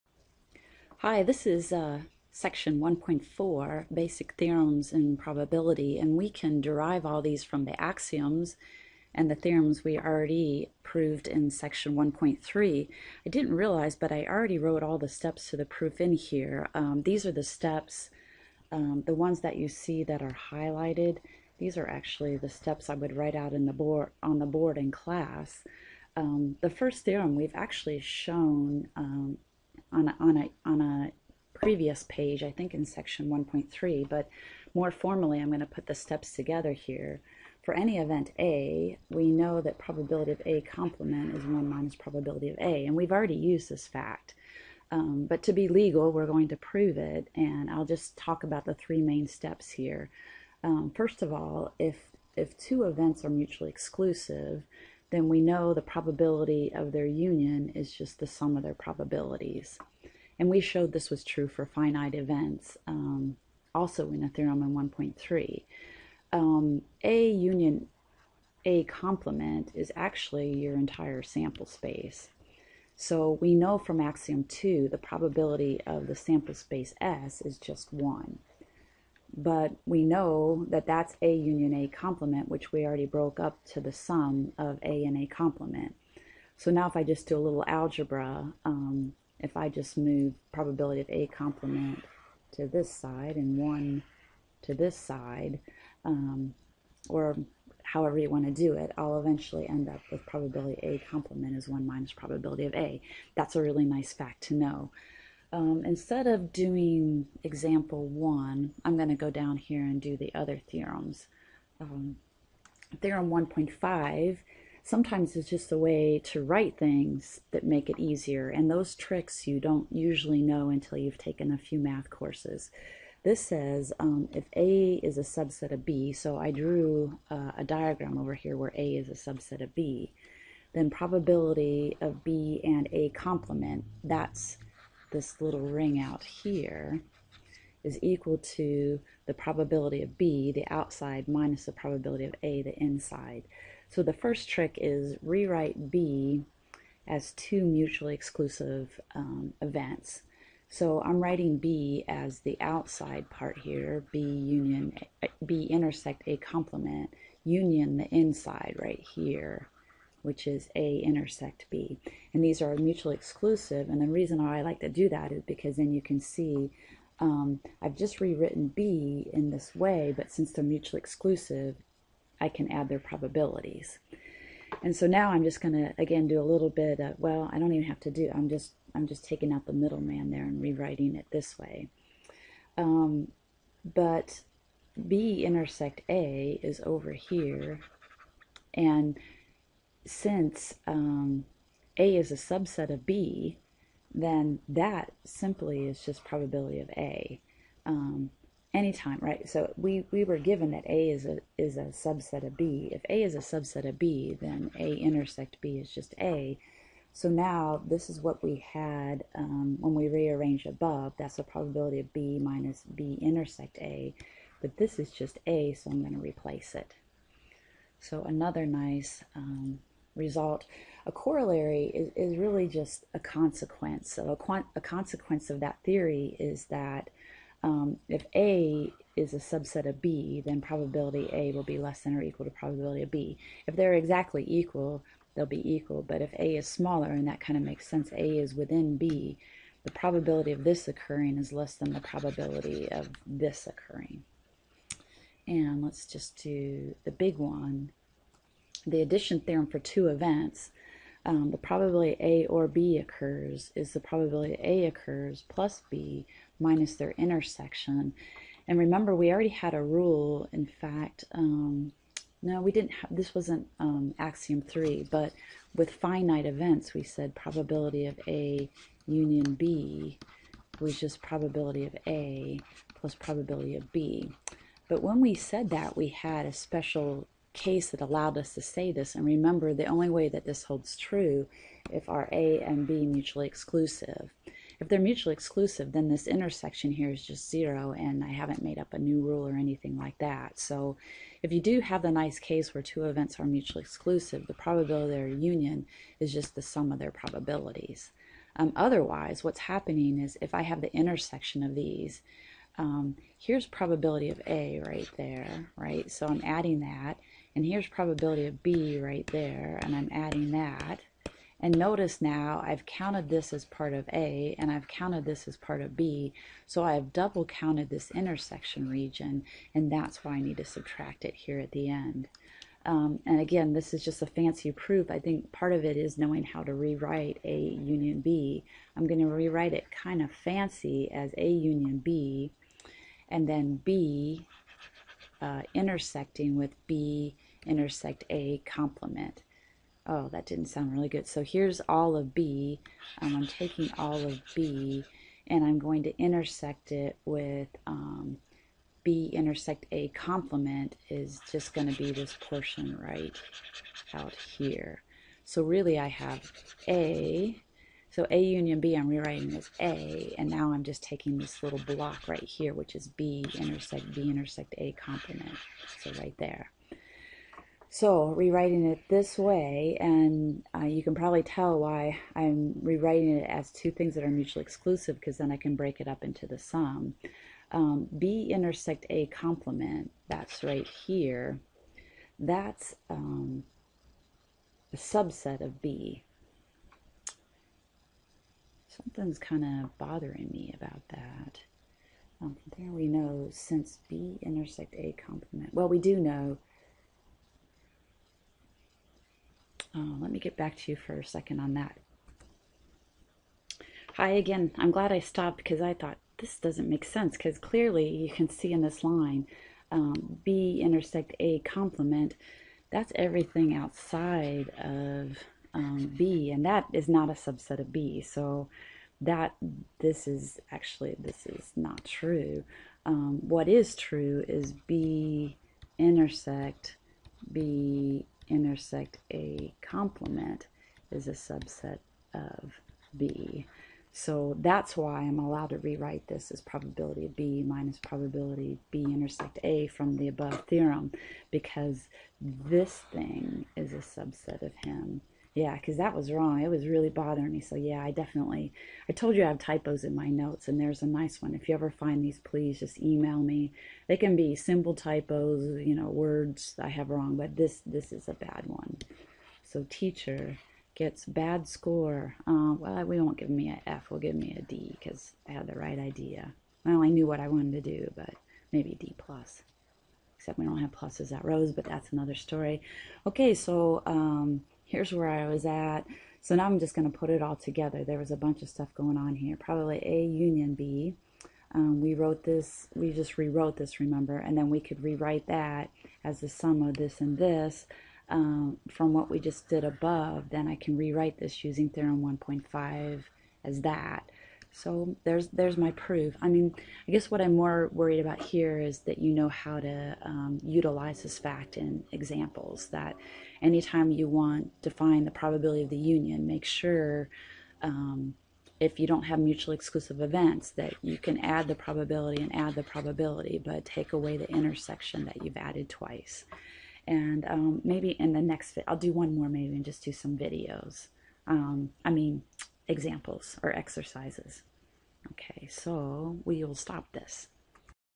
MA 381: Section 1.4: Basic Theorems of Probability - Proofs
A lecture showing several proofs of basic probability theorems.